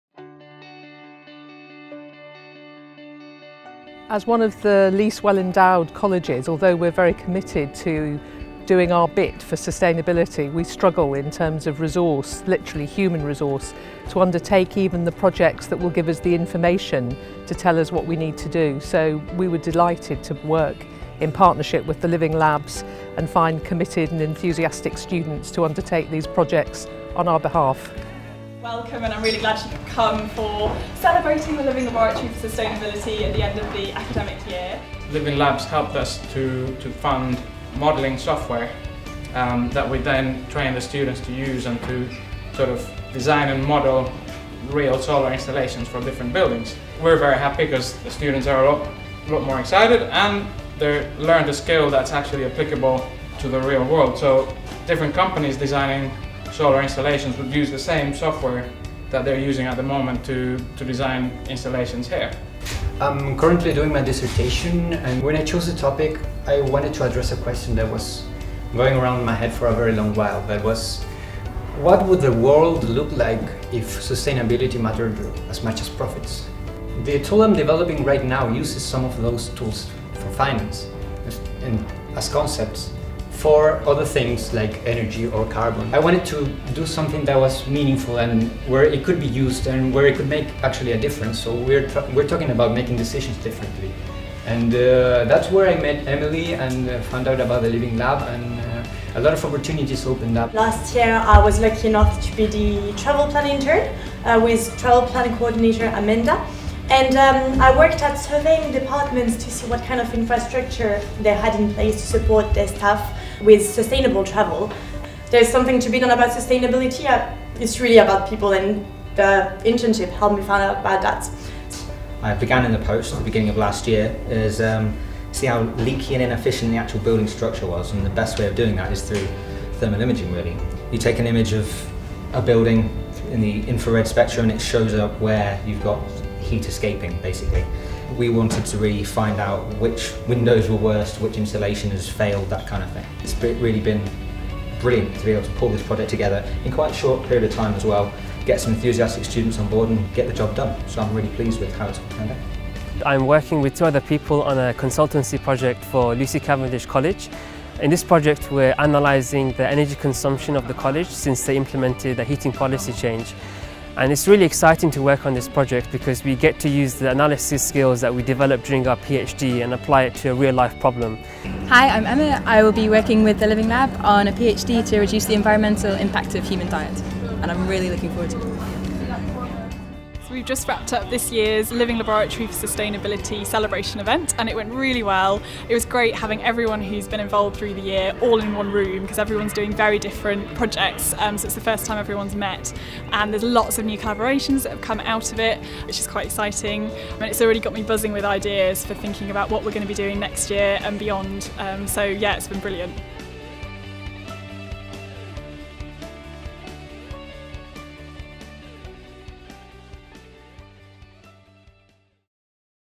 The Living Laboratory for Sustainability
The Living Laboratory for Sustainability provides opportunities for students to improve sustainability across the University through projects, internships and research. This short film showcases some of the activities and people involved over the last academic year (2015/16).